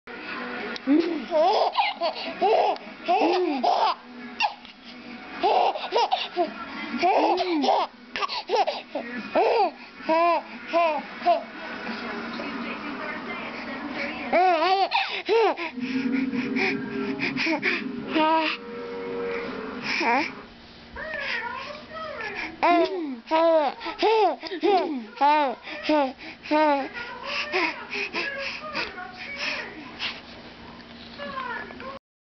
baby laughing
this is gage laughing at the camera!